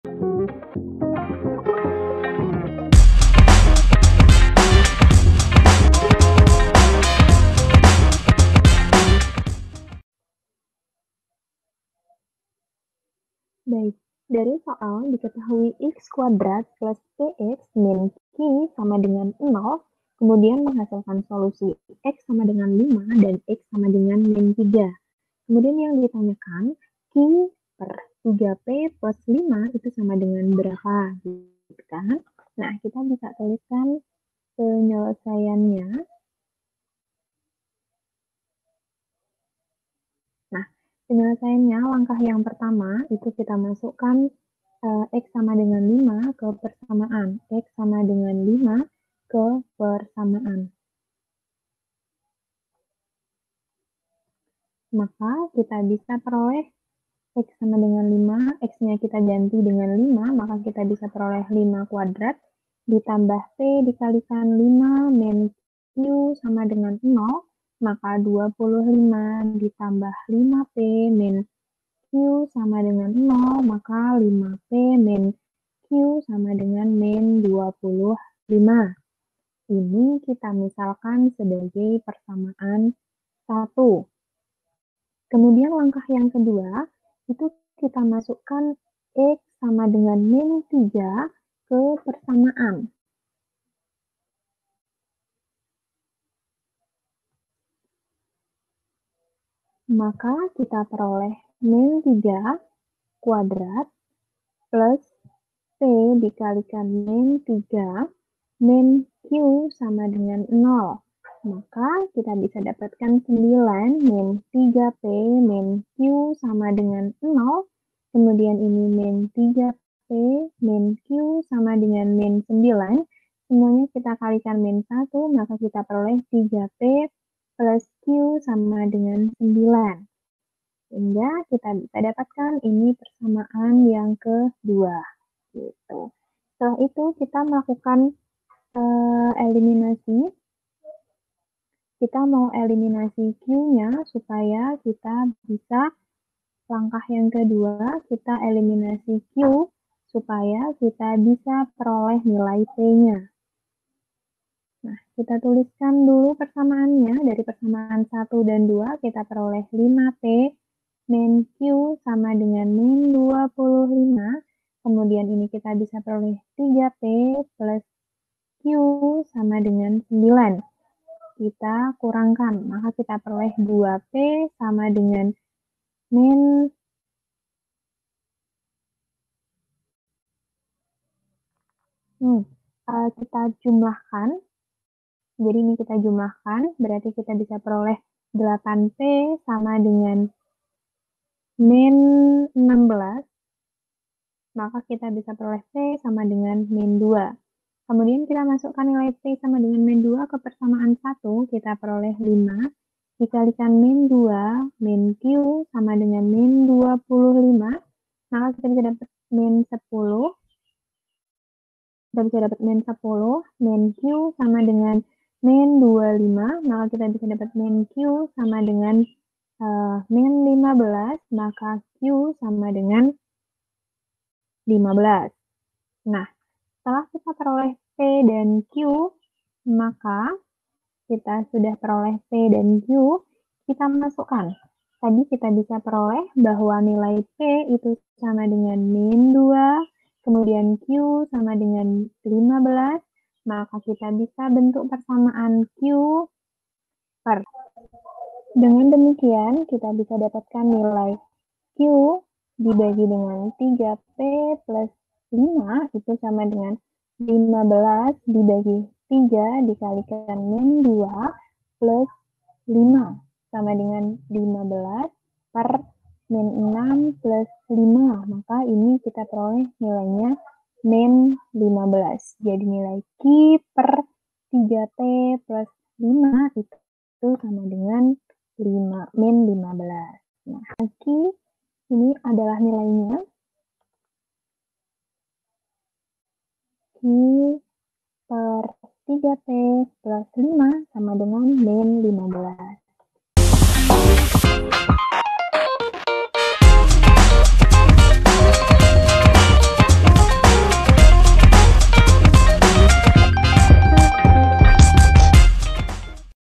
Penyelesaian x^2+px-q=0 adalah 5 dan - 3... | Erlangga X-Pres | Kelas 9 | Matematika
✅ Dalam video ini kita akan membahas: Penyelesaian  x^2+px-q=0  adalah 5 dan - 3.
Nilai dari \frac{q}{\left(3p+5\right)} adalah
A. 15
B. 3
C. -3
D. -15
Tema:
Kelas: 9
Mata Pelajaran: Matematika
Bab: 2
Buku: Erlangga X-Press UN SMP/MTs 2020 Matematika

👉 Buku yang telah tersedia:
{PLAYLIST_LINK}
Di mana kamu dapat menemukan kami?